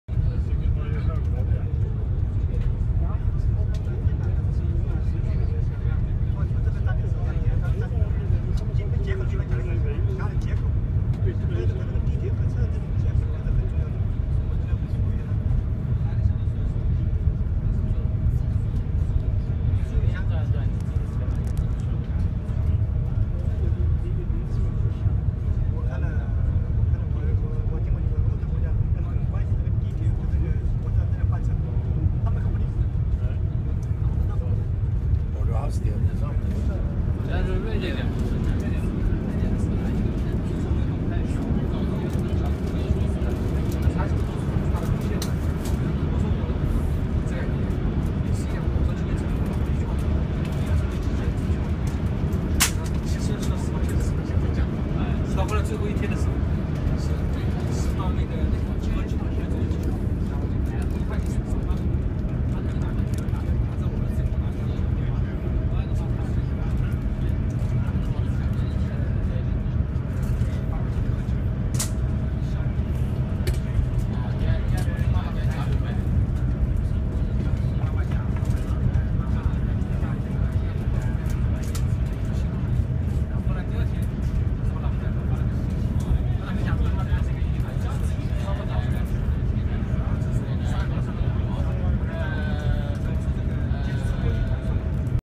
CRH380A High-Speed train, Shanghai-Nanjing
Lägg märke till 10-kronan. 300 km/h och inte tillstymmelse till krängning/skakning. Den andra personen som filmade 10-kronan gav upp efter 10 minuter, den rubbade sig inte en millimeter.
Banan är byggd på bro, dvs på prefabricerade broelement.